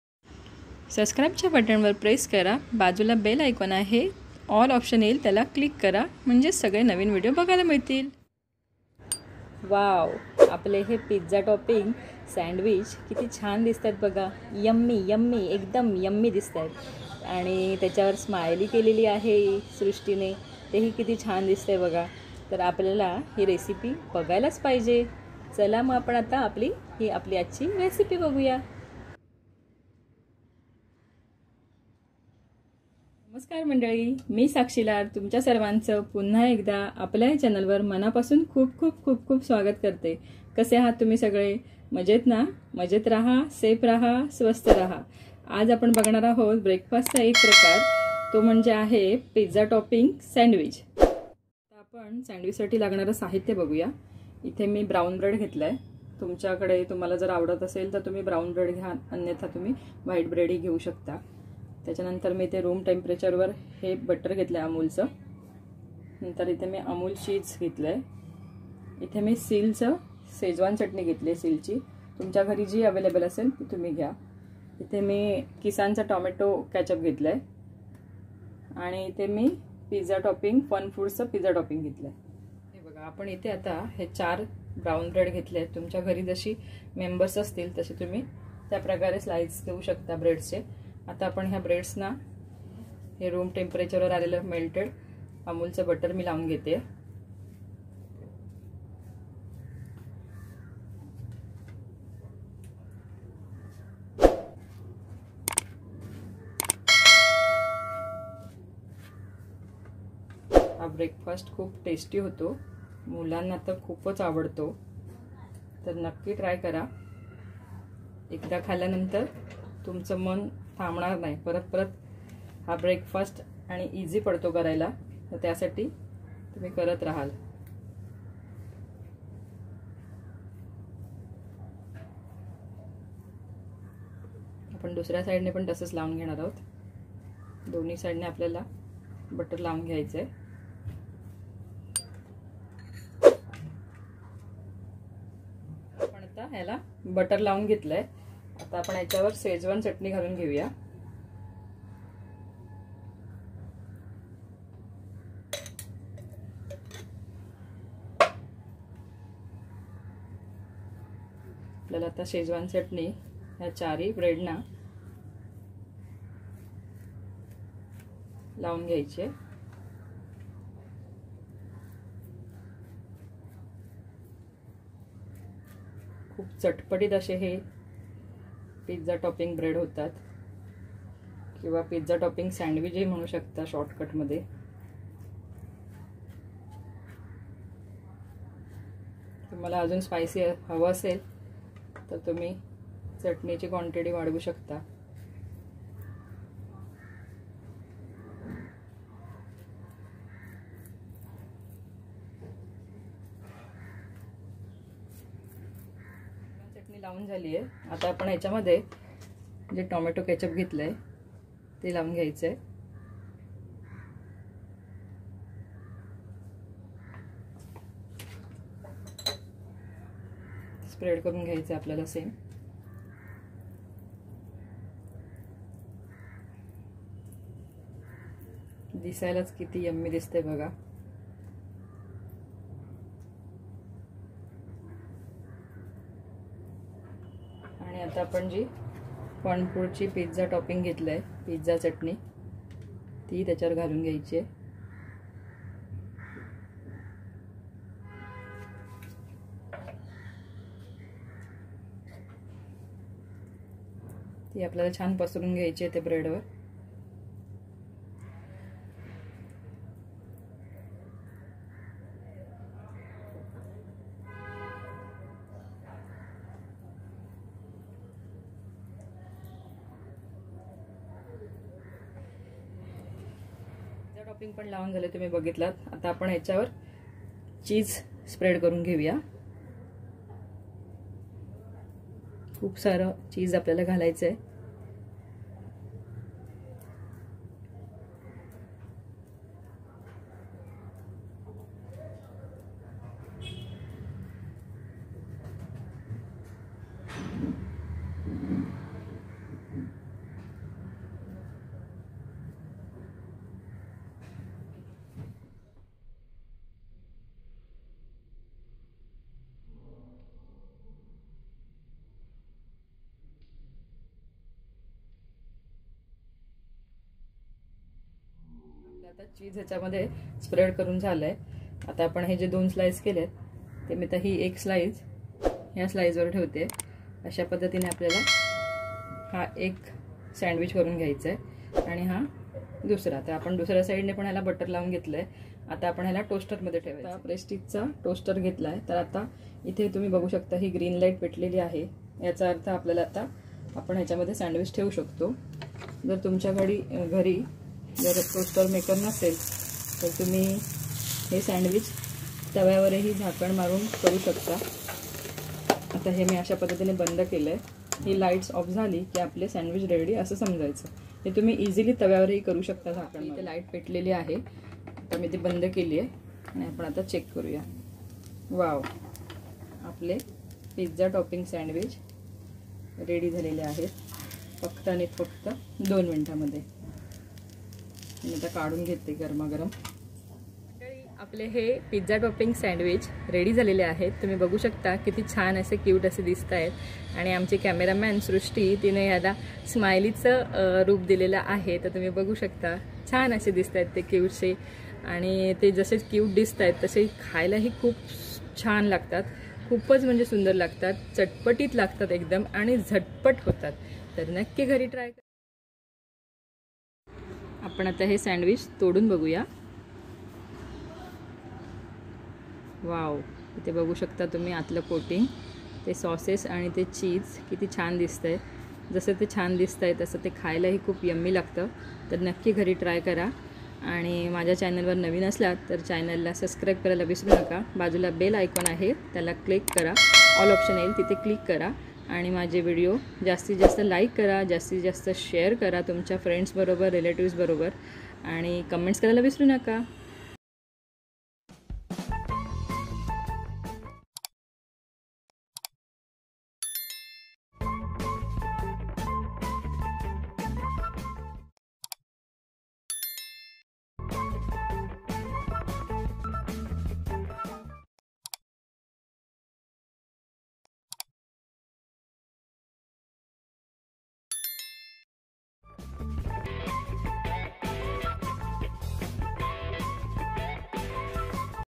Pizza Topping Sandwich😋 | Sandwich Recipes🥪 | Quick Breakfast|Toasted Sandwich| Recipe by Sakshi Lad
Ingredients for making Pizza topping sandwich :

1.  Brown Bread or White Bread
2.  3 spoon Butter
3.  1 Cube cheese
4.  Tomato sauce
5.  Pizza topping
6.  Schezwan chutney

You can use Sandwich maker or otherwise if u don't have Sandwich maker you can use Pan. From this 2 you can take anyone as per your choice

Keep watching